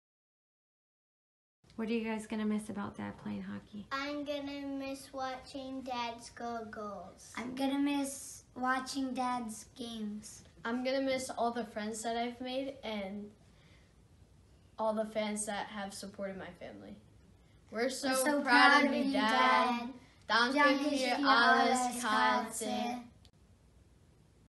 EC-KAC // Video Message Karter, Austin, Chloe Lundmark (Jamie Lundmark Night)
Rotjacken count down the days to Jamie Lundmark Night on January 27th with video messages from our number 74's family and former teammates. Part 2: Karter, Austin and Chloe Lundmark.